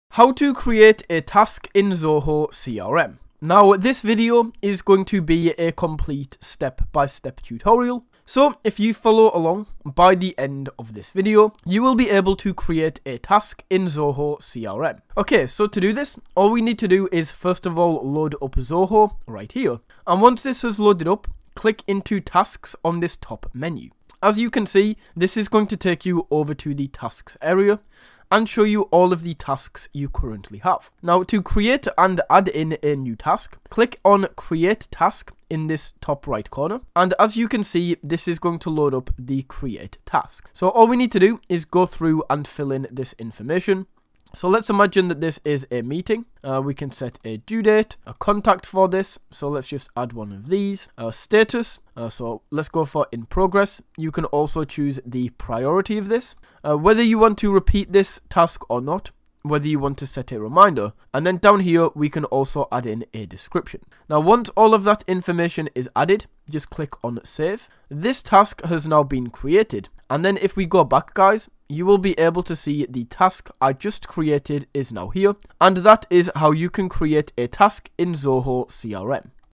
How To Create Task in Zoho CRM
In this tutorial I will show you How To Create Task in Zoho CRM